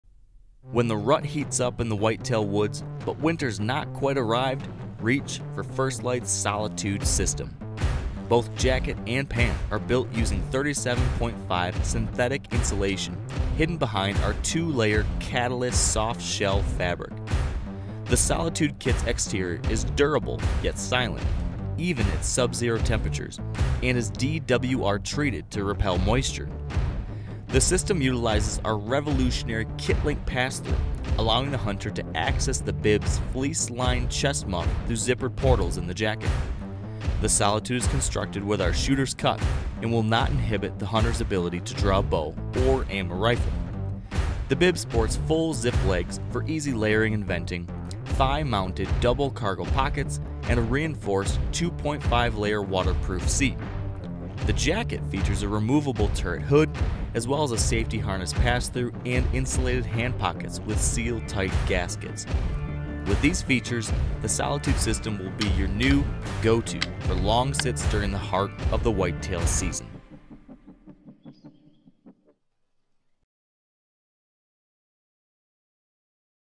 The Solitude Insulated System
Dead Silent. Fully-featured. Seamless integration. A kit designed from the ground up to span the heart of your season. Worn on their own or combined to maximize the kit's true performance, the Solitude Kit offers never-before-seen versatility for the whitetail hunter. Utilizing our revolutionary, KitLink pass-thru on the Solitude Jacket you'll always be able to access the Solitude Bib's chest muff pocket, and all the gear you keep in it.

The Solitude kit combines all the performance characteristics of our proven whitetail styles into an integrated system ideal for mid-season sits. Both jacket and pant are built using 37.5 synthetic insulation hidden behind our silent, brushed 2-layer Catalyst softshell fabric, to regulate your body temperature regardless of the weather, while you wait on the buck of your dreams.